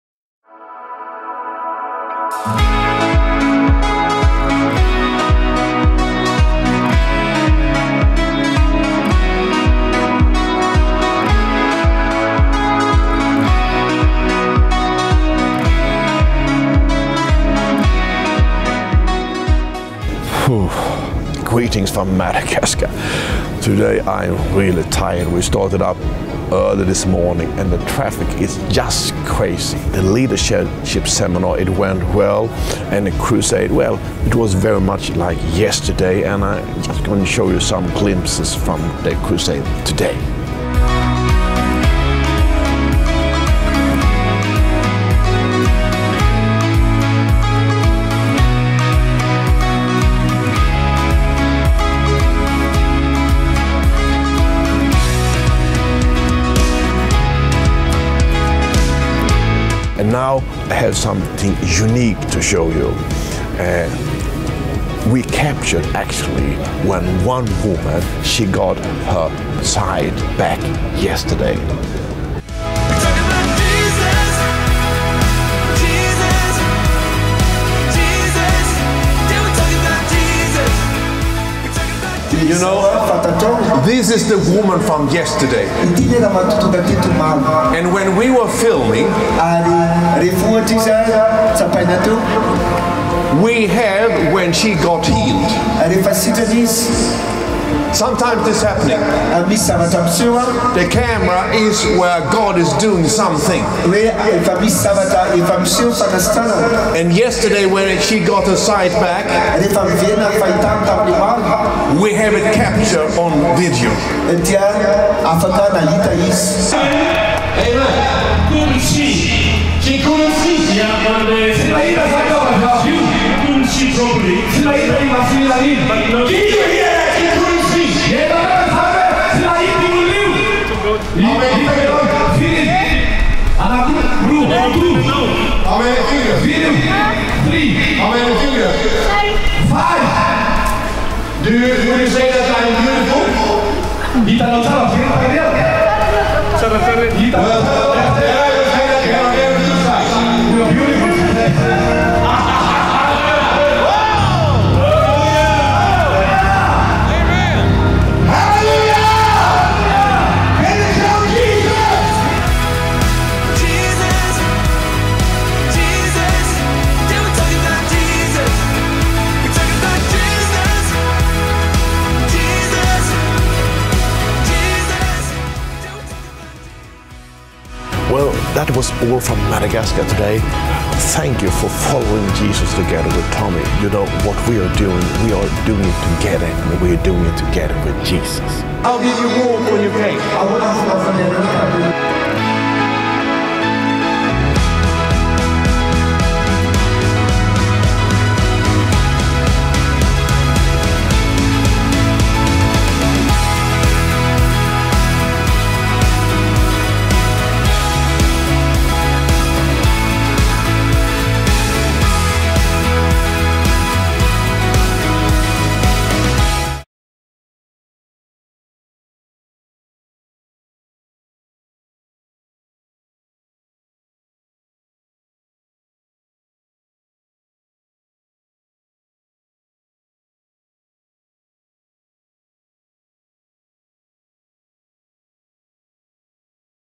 VLOG Madagascar Day 2
Here is the video report from Day 2. We happened to film a woman in the audience right during the moment she got her sight back. That was unique! At the end of the video you’ll see the girl with the broken foot show the difference between how she walked when she came, and how she can walk now that Jesus has healed her.
These are incredible miracles that only Jesus can do.
I just got back from the leader seminar. We had a double theme today: “Give No Place to the Devil” and “Expand Your Territory.”